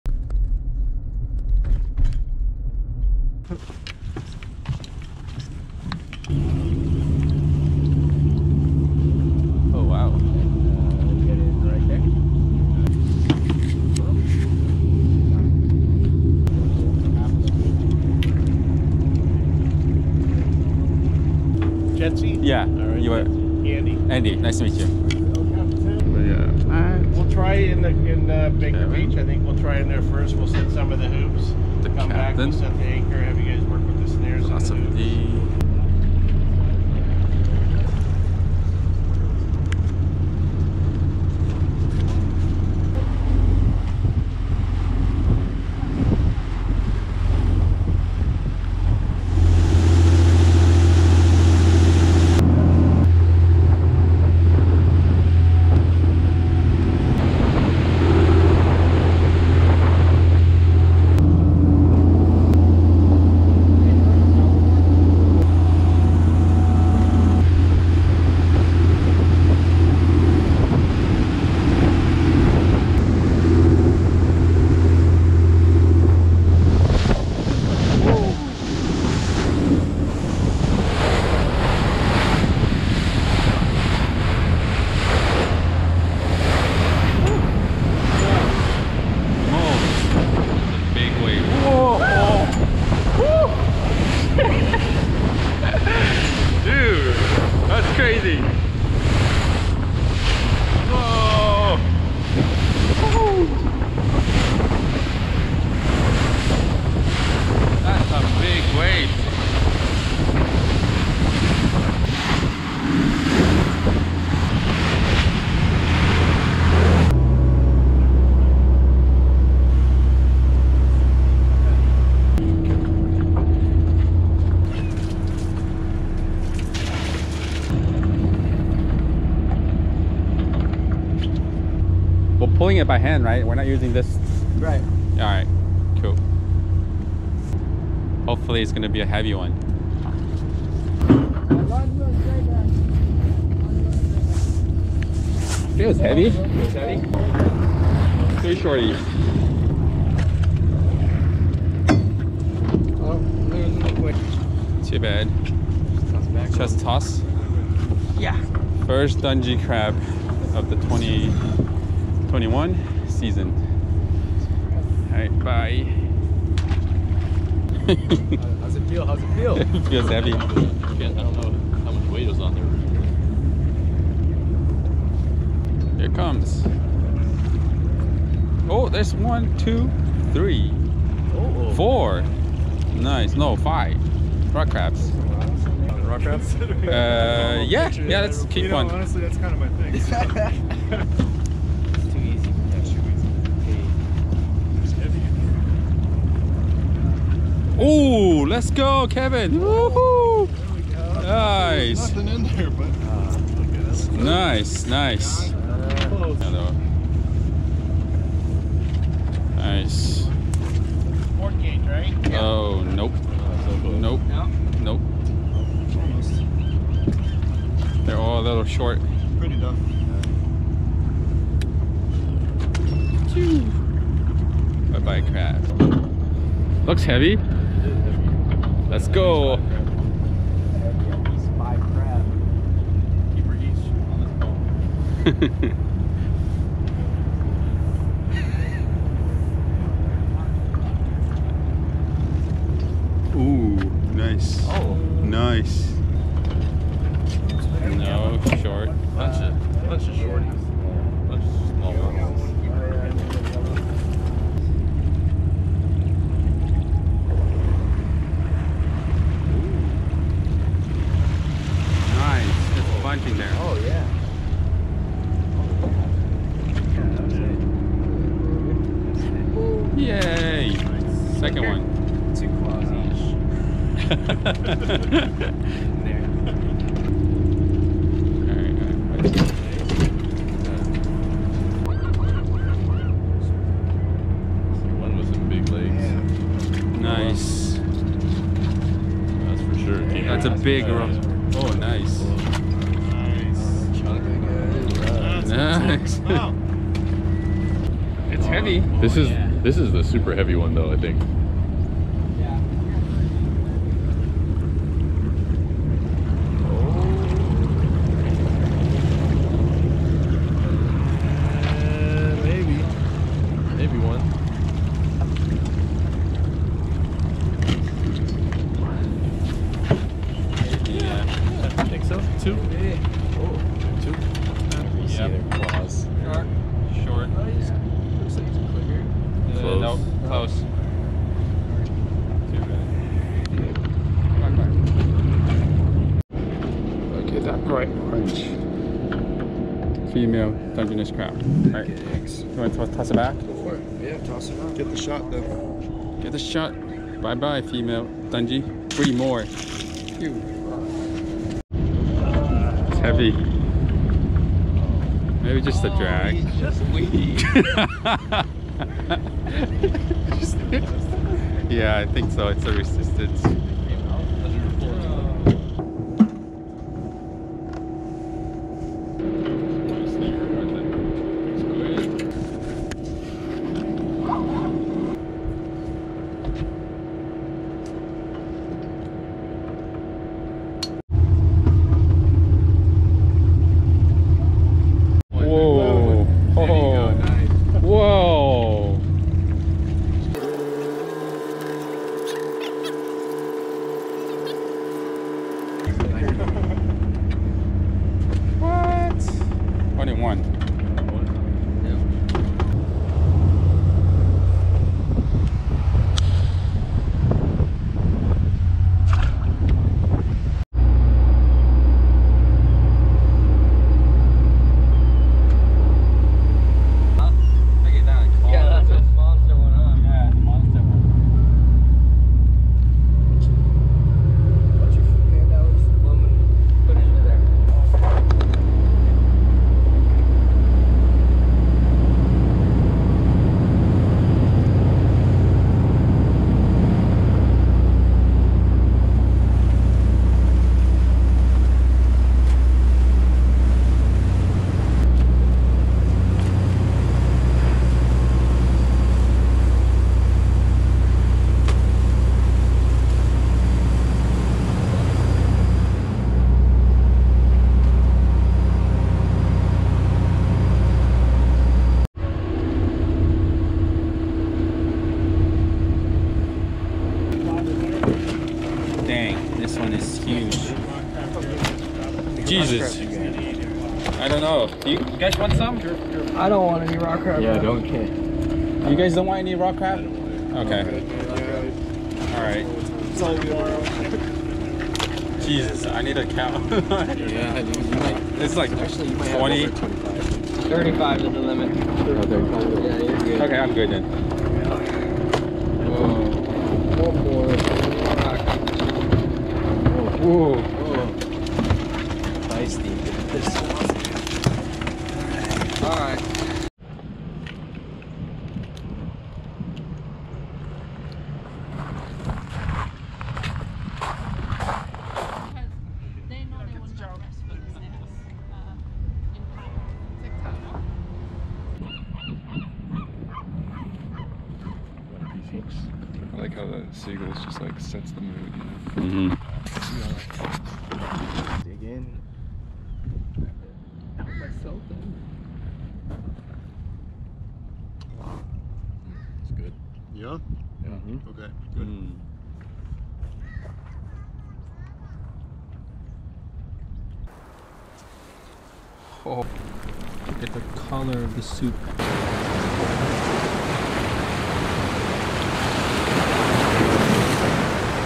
Crab Fishing With Catch N Cook California, Fisherman's Life and Philosophy D!
Thanks for the invite from Matt @FishermansLife , I was able to join with @catchncookcalifornia1574  and @philosophyd  on the Pacific Pearl operate by captain Andy and his crew I got my first ever dungy crabs in the  2021-22 Dungeness crab season. It was a lot of fun and i get to keep if not all but most of the rock crabs and man it was a feast.